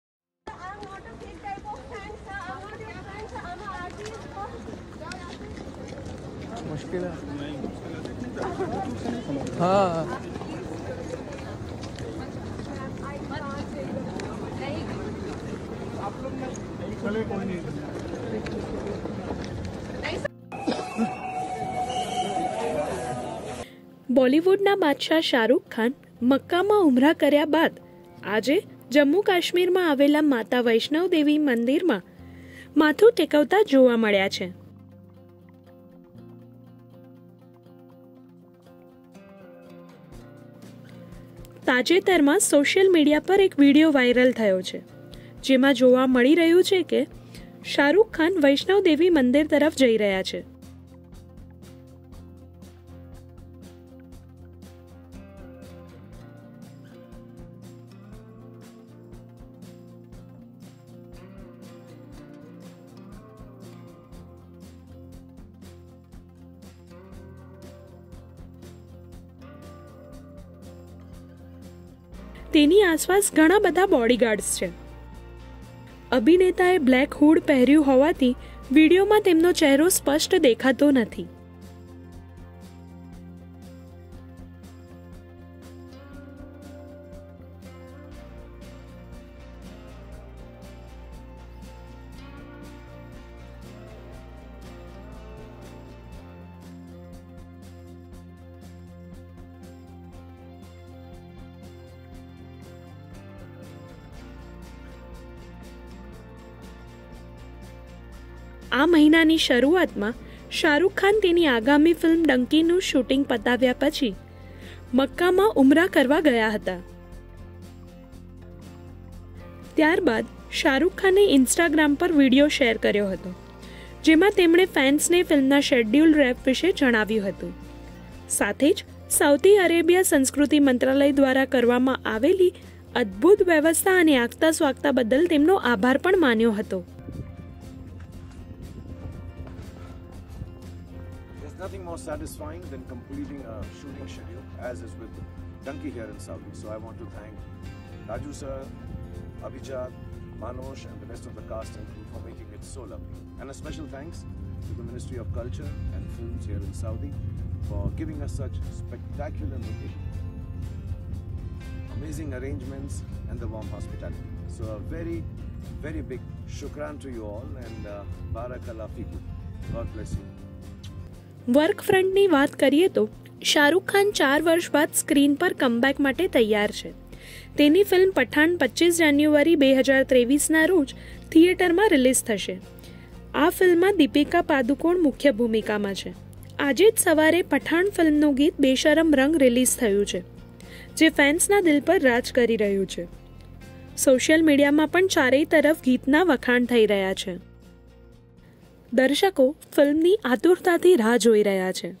After Mecca, Shah Rukh Khan Visits Vaishno Devi Temple In Katra
Shah Rukh Khan, after visiting Mecca, was spotted visiting Vaishno Devi temple in Jammu and Kashmir. In a video, Shah Rukh can be seen surrounded by his security guards, walking towards the temple. The actor’s face is not visible as he is wearing a black hooded jacket. Earlier this month, the actor was spotted performing Umrah in the city of Mecca after the schedule wrap of his upcoming movie Dunki.